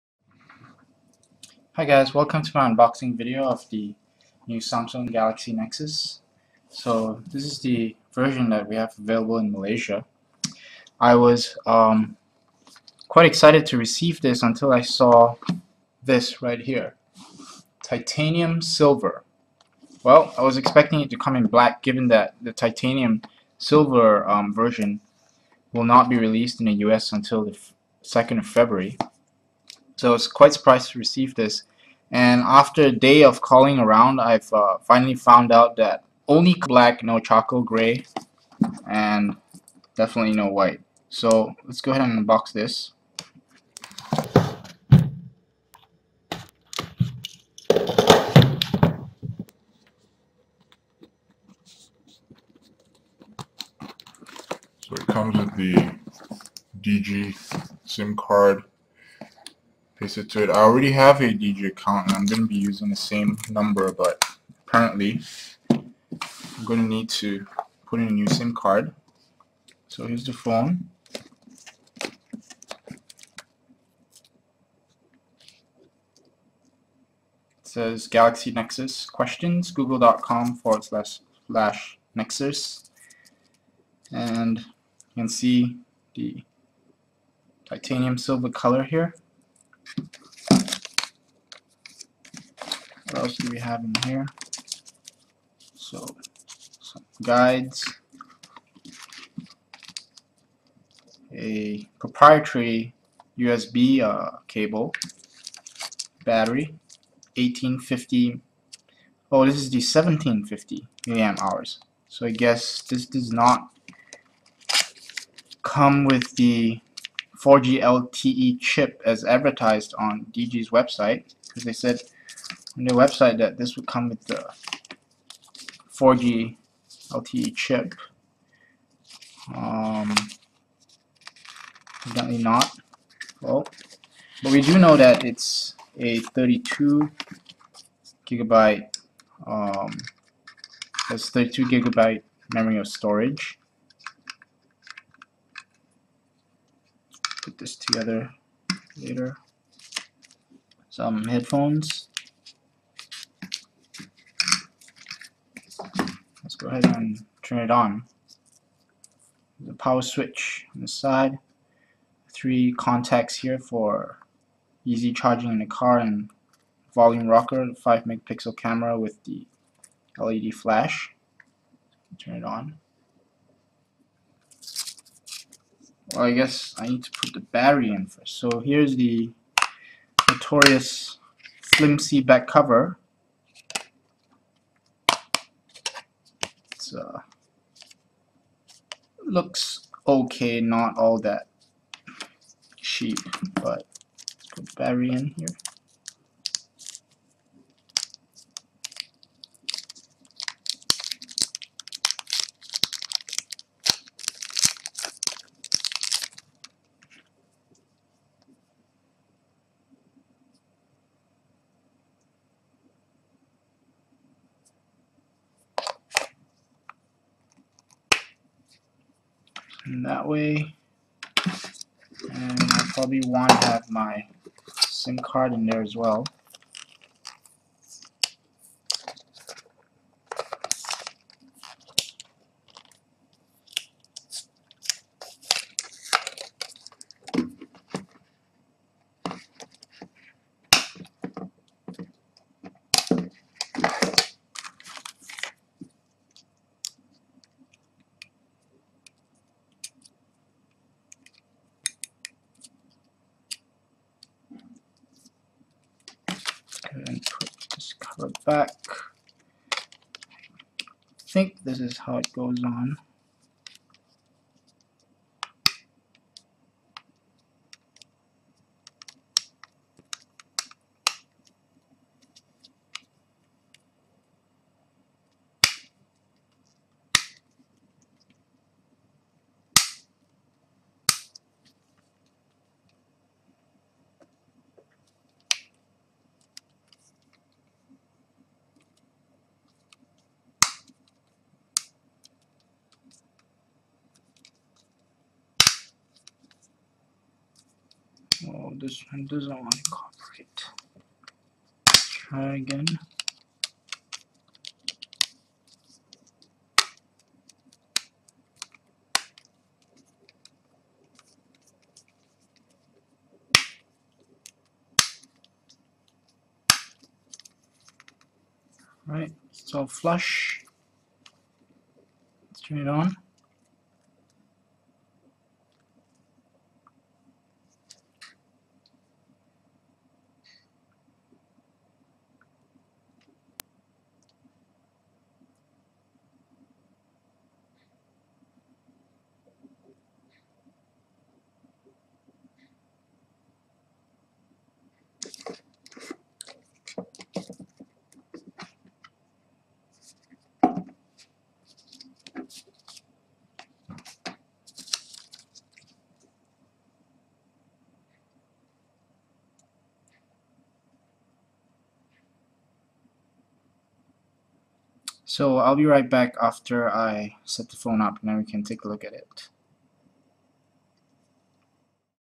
Samsung Galaxy Nexus Unboxing (Titanium silver)
From DiGi Malaysia. As expected a few surprises. Only Titanium Silver is available in Malaysia. Tech specs link from DiGi's website specified the LTE version with 32GB of storage, but as I discovered, this phone has not LTE chip and comes with only 16GB of SDcard memory. Also, the battery is 1750mAh, not 1850mAh as advertised.